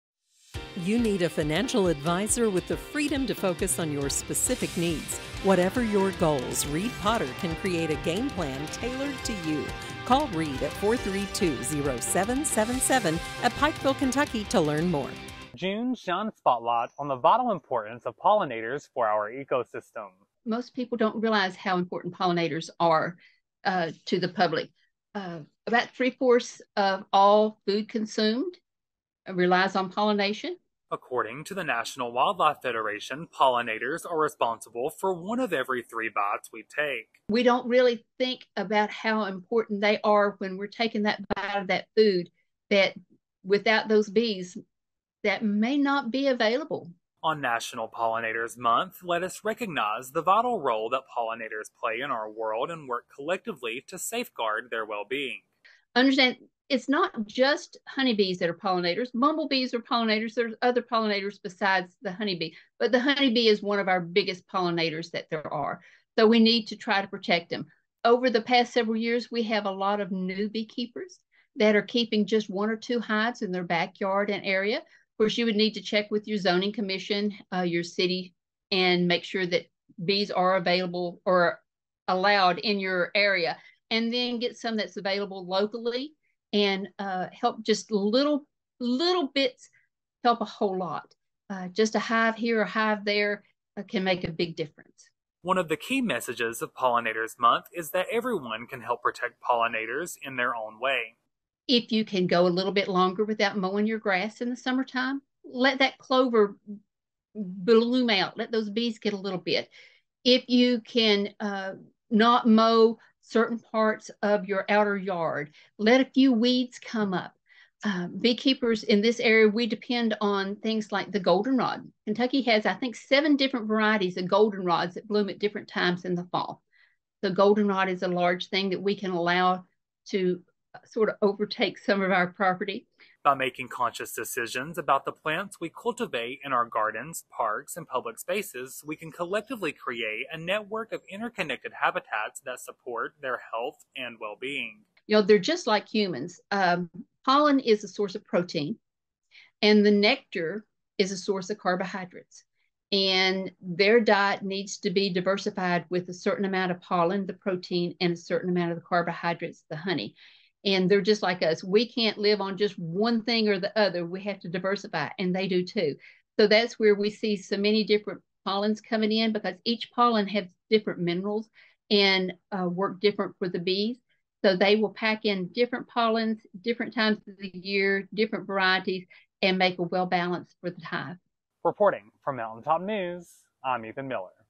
June Shines a Spotlight on the Importance of Pollinators for Our Ecosystem
June is National Pollinators Month, and we take a look at the important role they play in our ecosystem in honor of it.